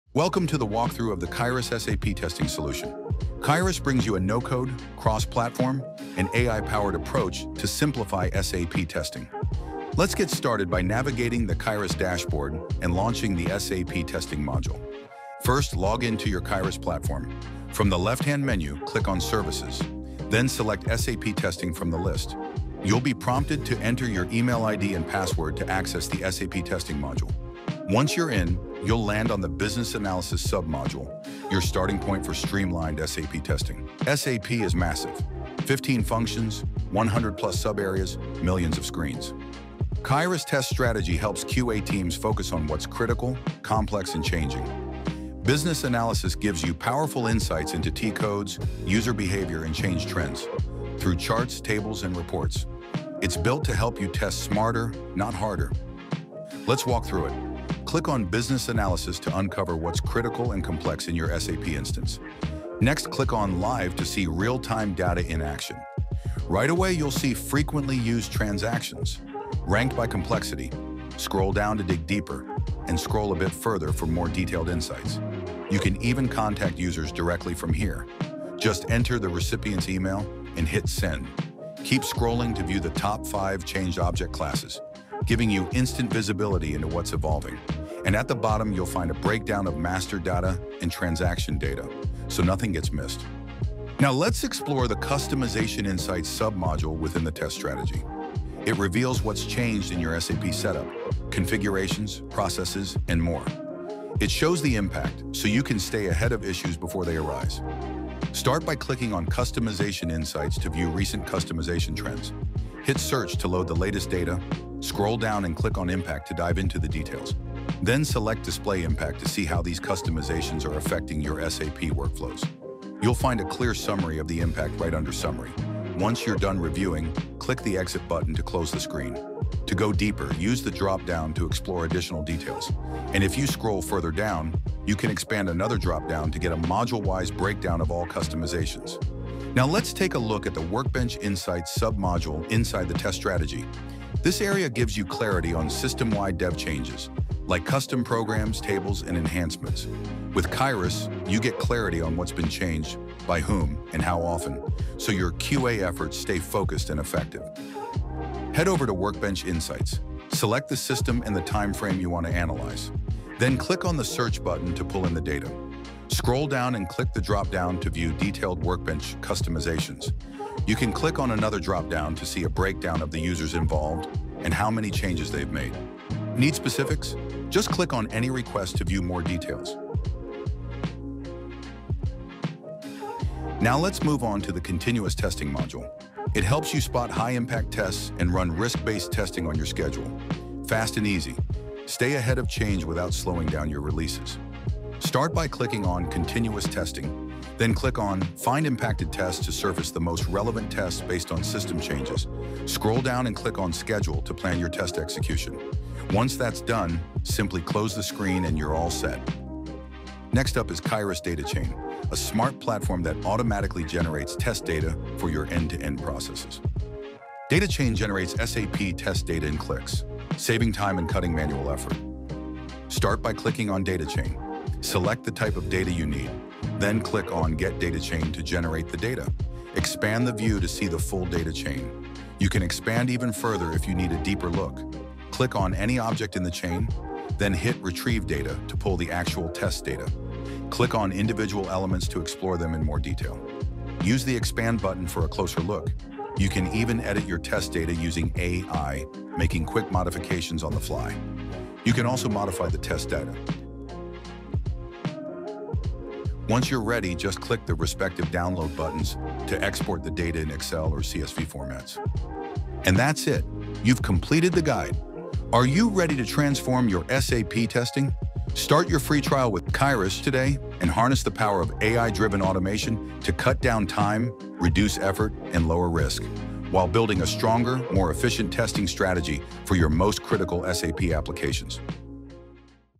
Qyrus SAP Testing Walkthrough | Boost ROI & Cut QA Risks
Struggling with complex SAP testing?

Discover how Qyrus simplifies SAP test automation with a no-code, cross-platform, AI-powered approach—designed to maximize ROI and minimize testing risks.

→ Accelerate SAP release cycles
→ Eliminate manual testing bottlenecks
→  Reduce defects with intelligent test coverage
→  Achieve faster time-to-value with scalable automation

Whether you're testing SAP S/4HANA, ECC, Fiori, or hybrid environments, Qyrus helps QA teams move faster with confidence.